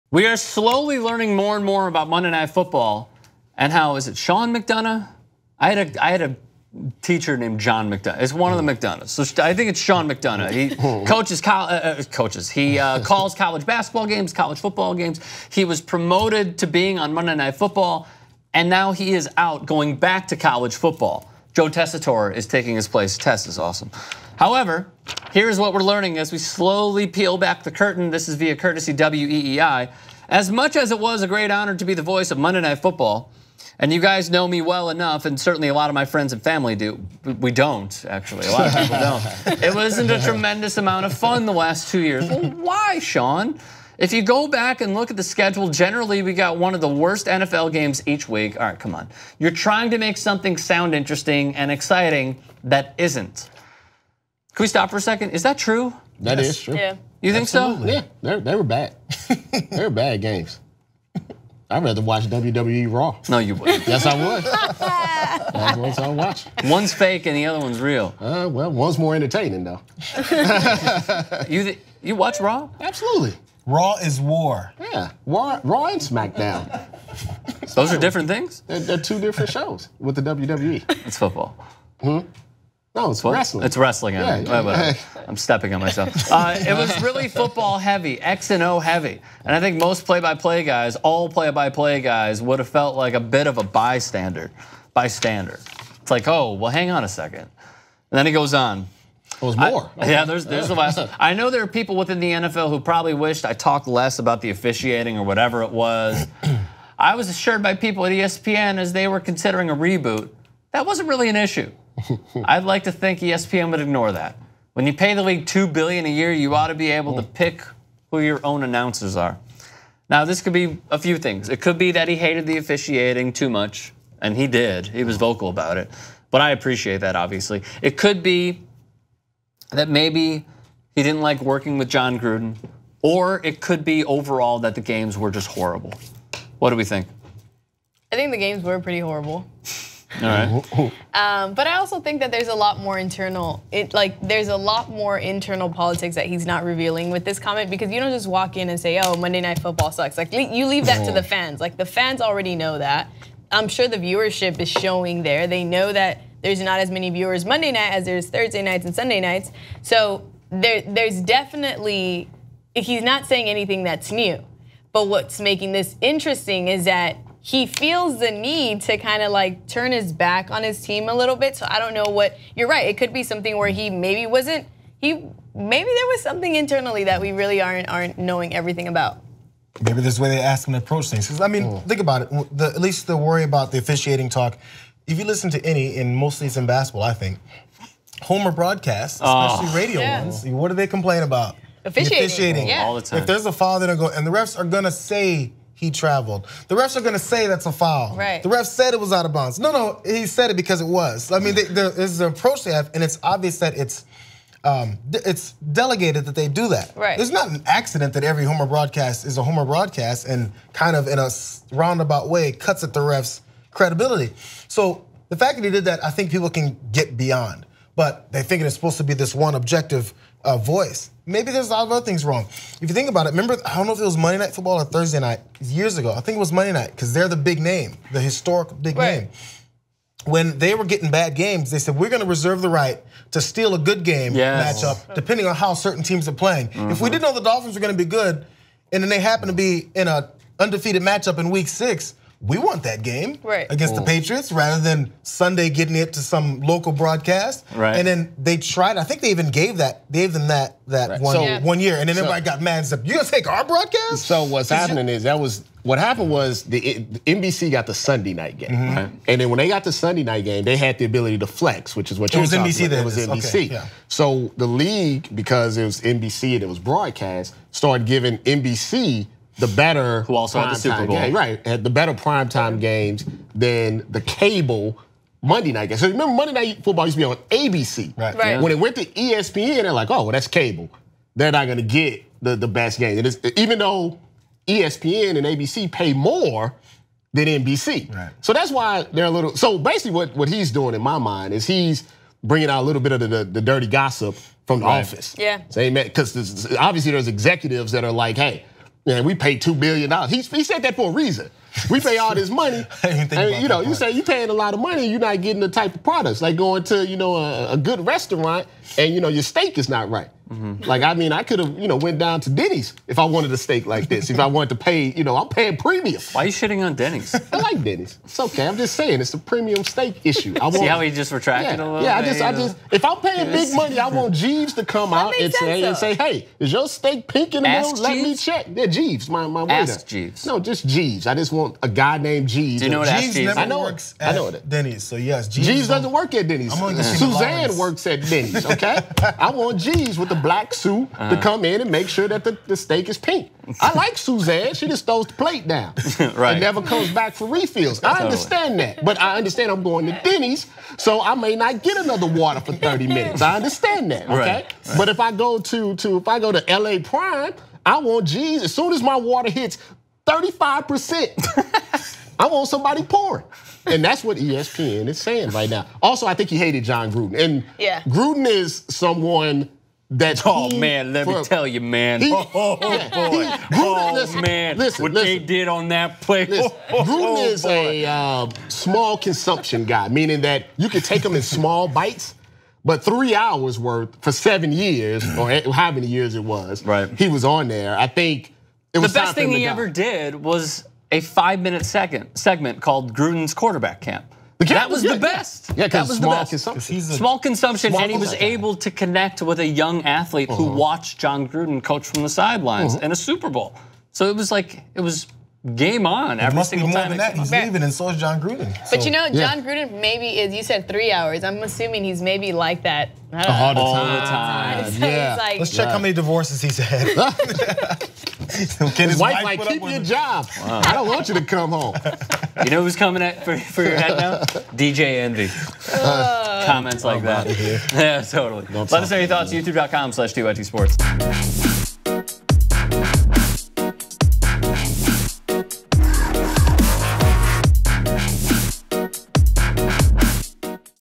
ESPN Broadcaster Thinks Monday Night Football SUCKS
Does MNF suck? A former broadcaster sure thinks so! Rick Strom, Robert Littal, Denise Jones, and Jayar Jackson break it down.